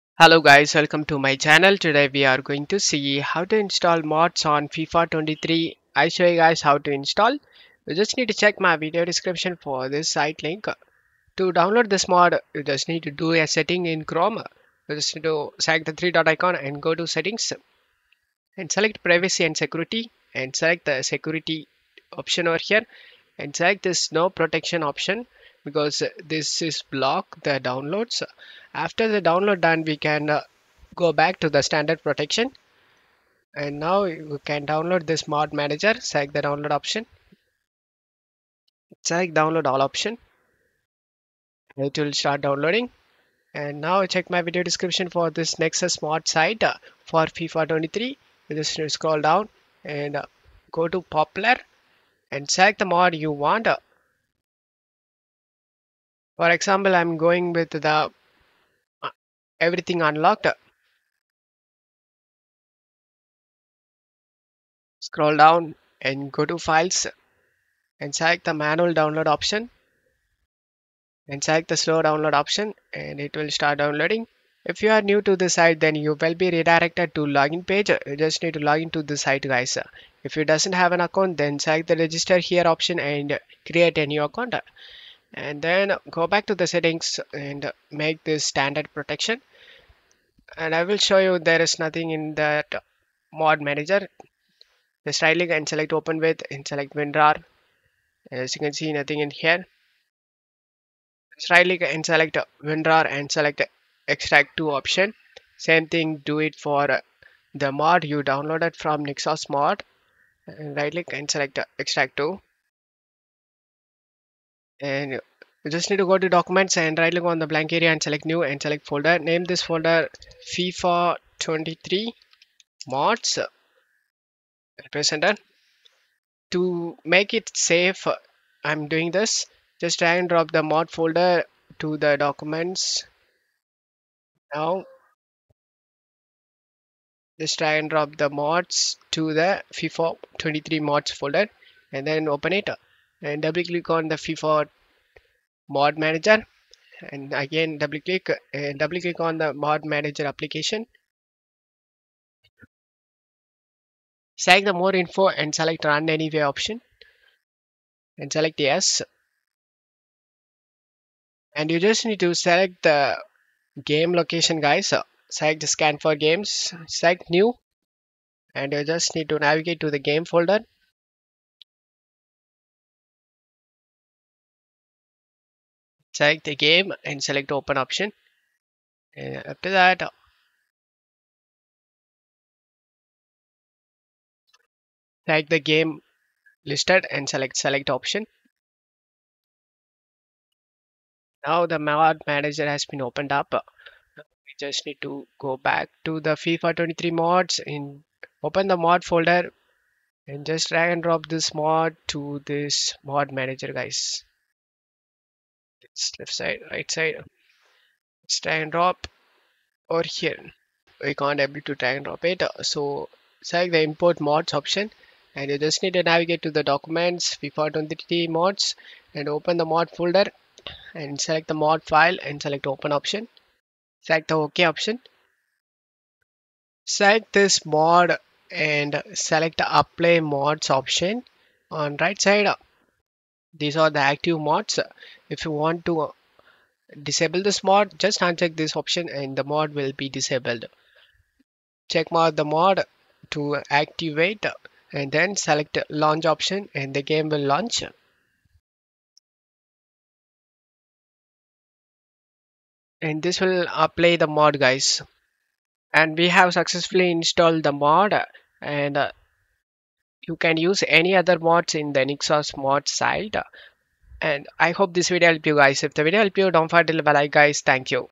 How to Install FIFA 23 Mod Manager (FIFA Editor Tool Setup Guide | 2025)
Learn how to install FIFA 23 Mod Manager and set up the FIFA Editor Tool (2025 update) step-by-step. This guide helps you fix common setup errors, import mods easily, and manage your FIFA 23 gameplay without issues. Whether you're editing teams, kits, or gameplay mods, this tutorial covers everything — from installation to launching mods safely. Perfect for beginners and modders who want smooth performance in FIFA 23.

Link Used
Mod Manager:

Steam file Path:
C:\Program Files (x86)\Steam\steamapps\common\FIFA 23\

EA app File Path:
C:\Program Files\EA Games\FIFA 23\